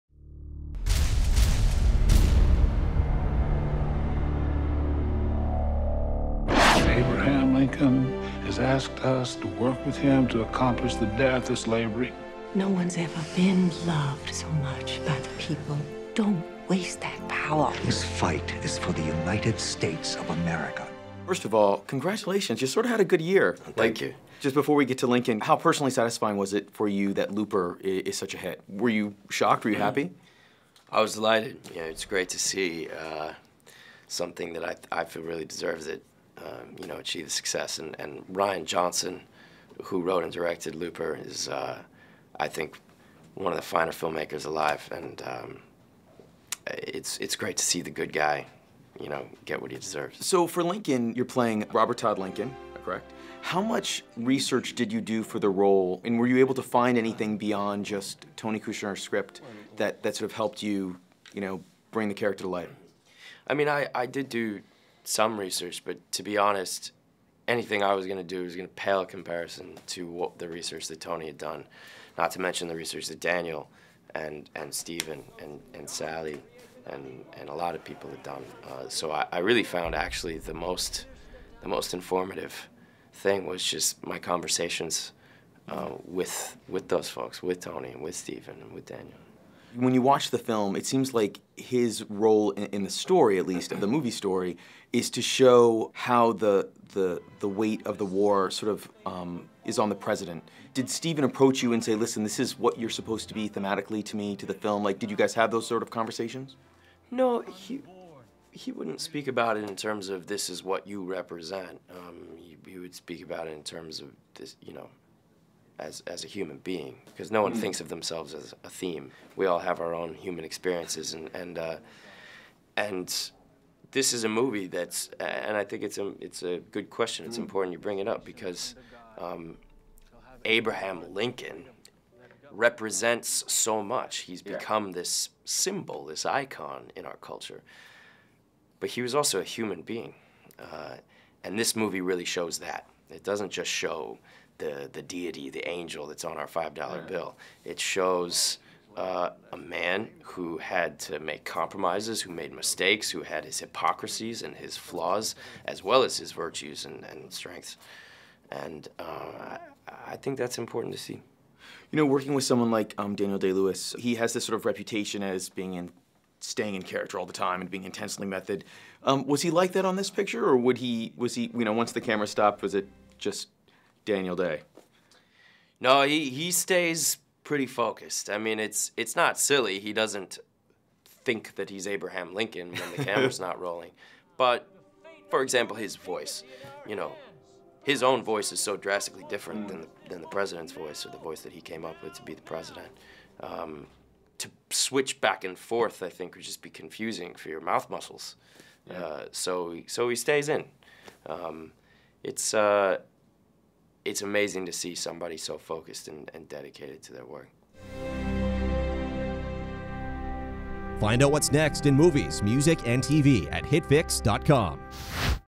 Lincoln - Interview with Joseph Gordon-Levitt
Joseph Gordon Levitt discusses his charachter Robert Todd Lincoln, the human side of Abraham Lincoln, working with Steven Spielberg and Daniel Day-Lewis